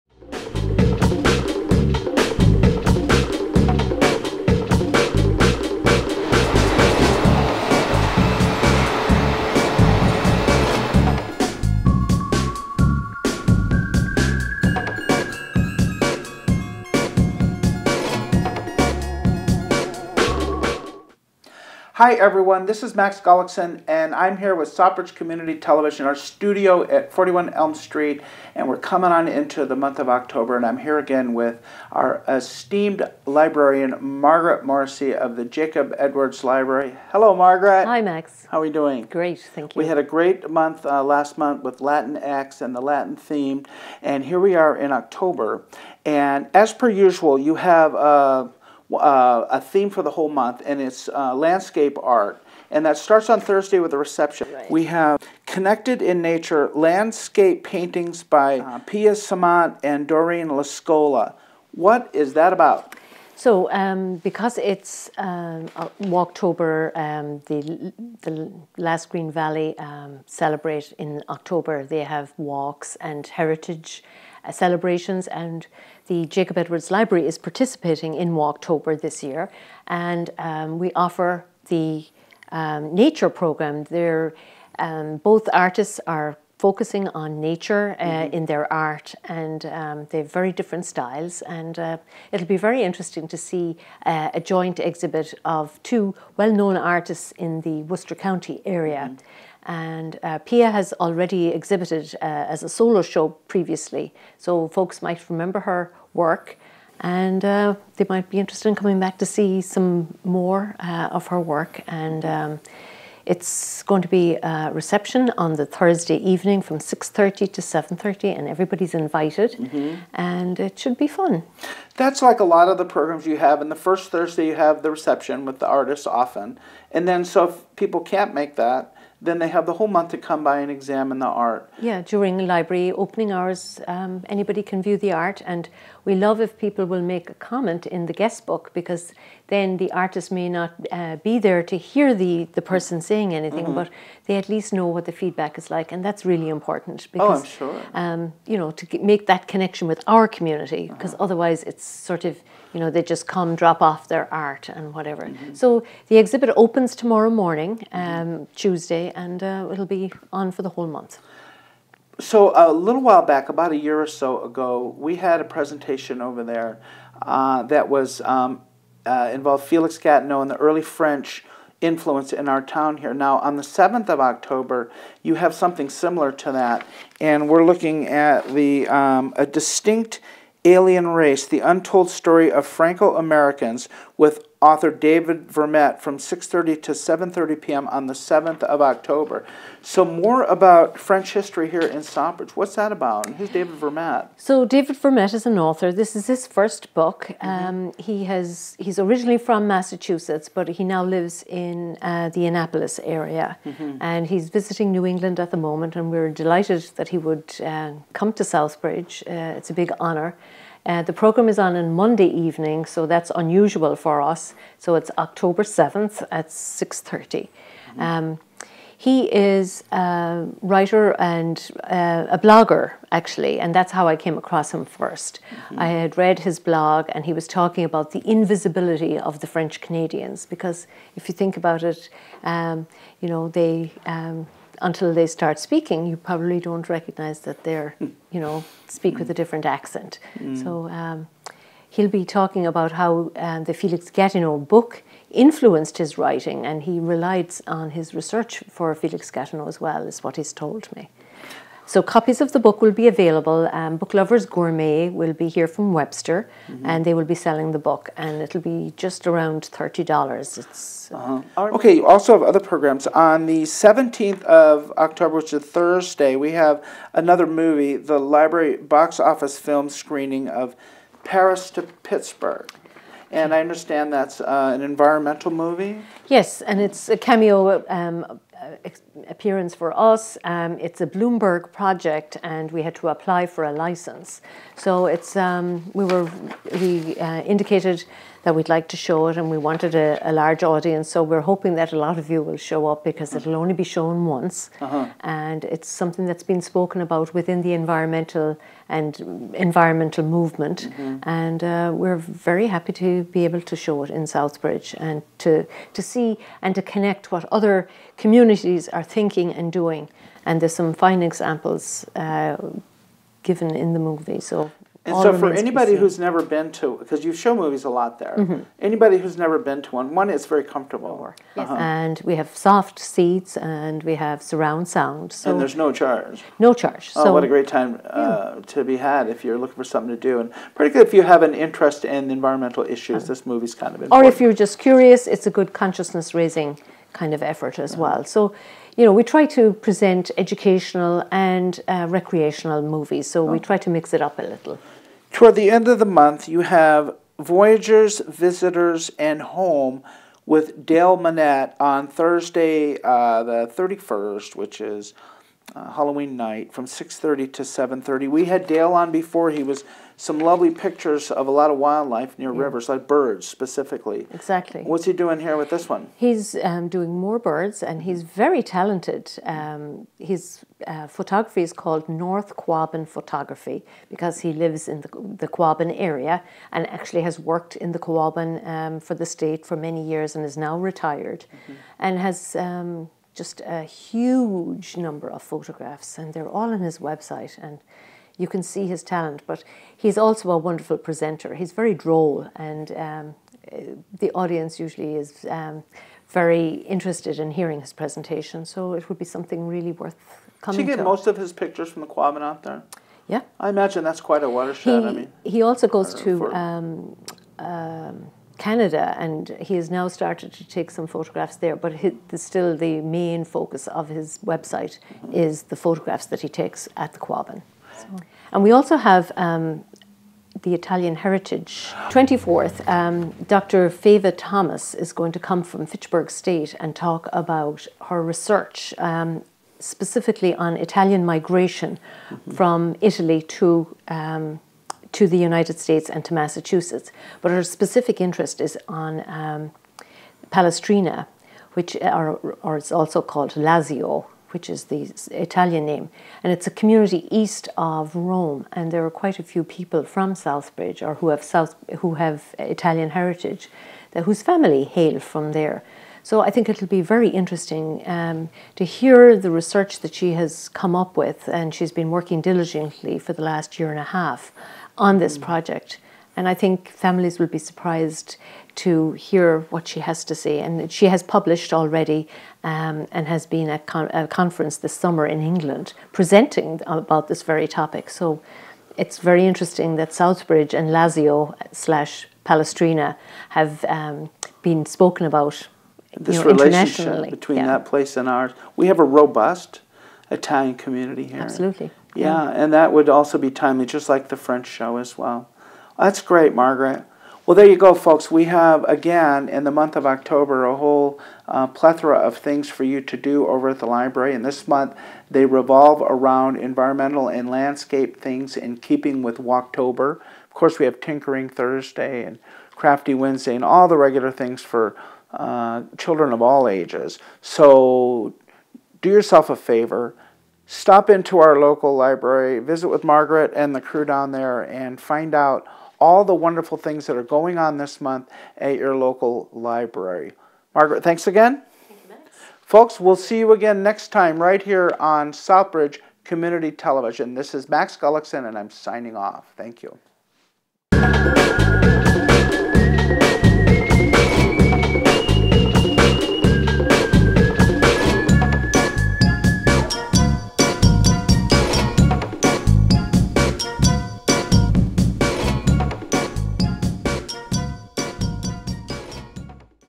Jacob Edwards Library events in October!
Watch as Max interviews Margaret about all the upcoming events happening at the JEL during October of 2019!